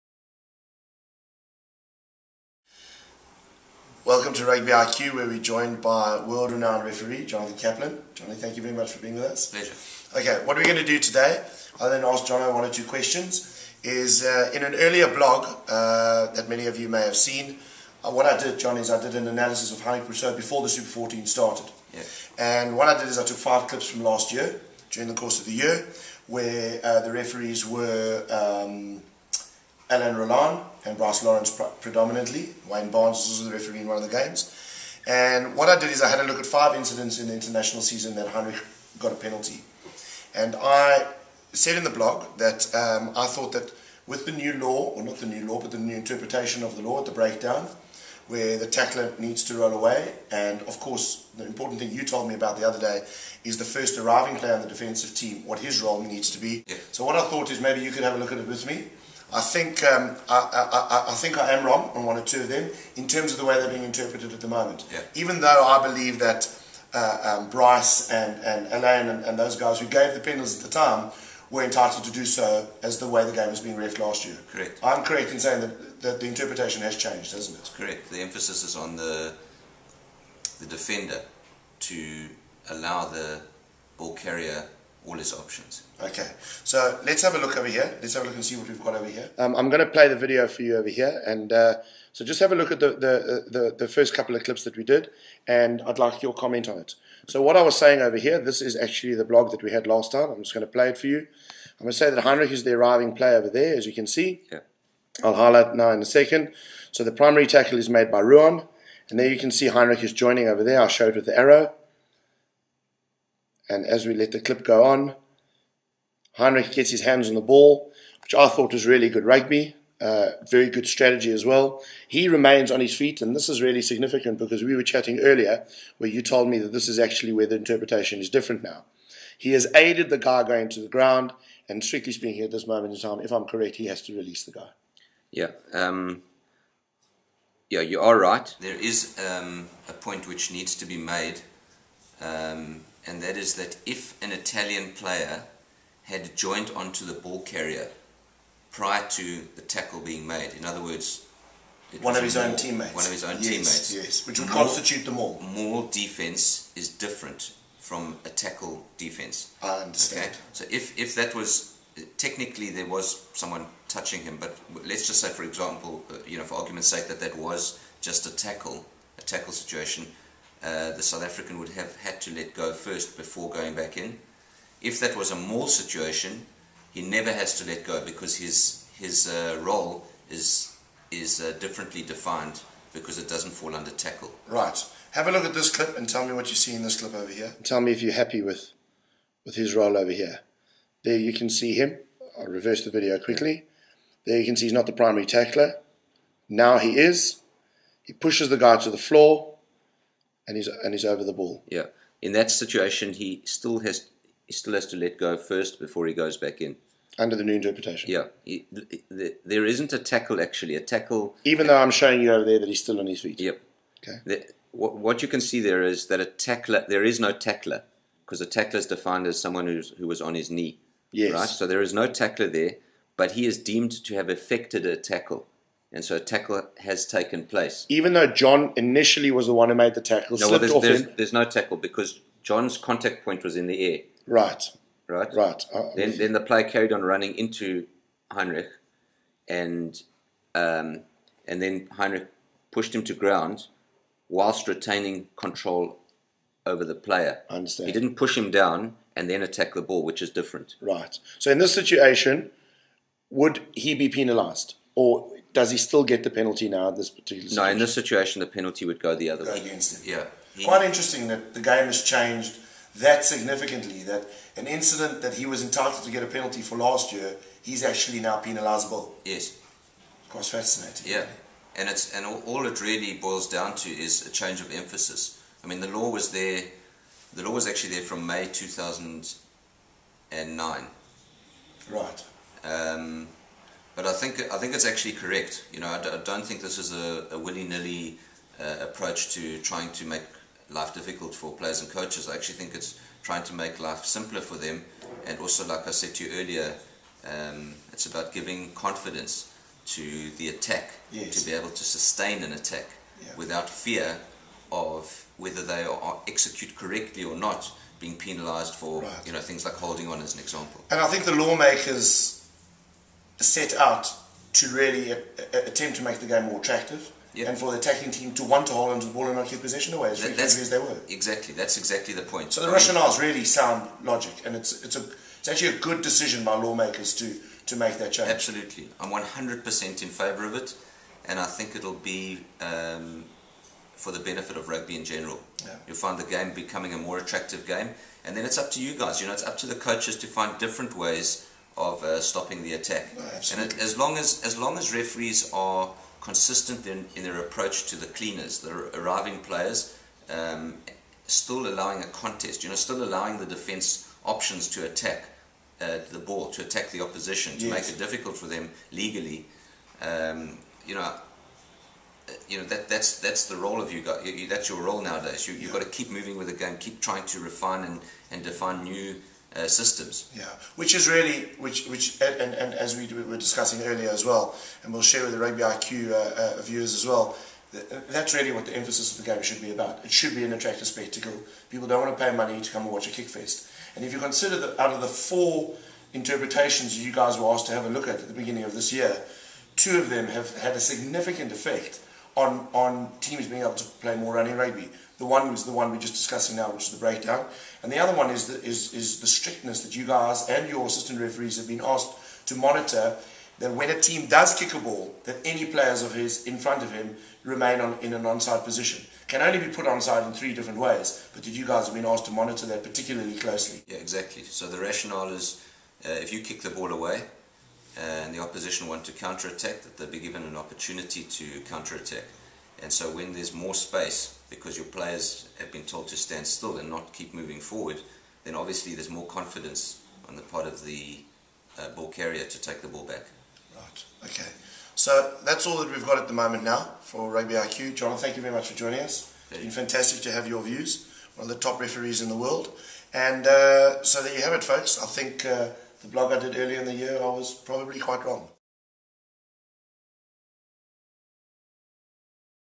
rugbyiq.com blog with Jonathan Kaplan
In this latest rugbyiq blog, Springbok assistant coach Gary Gold chats to the world's most experienced referee, Jonathan Kaplan, about a previous blog, where we discussed the changes in law interpretation.

Jonathan shares some fascinating views of the role of the tackler, what constitutes a tackle and the difference between the tackle and a maul, and his view of the responsibility of the coach in being able to adapt to the change.

This video was edited and made with Sportscode, the leading software in any sports video analysis.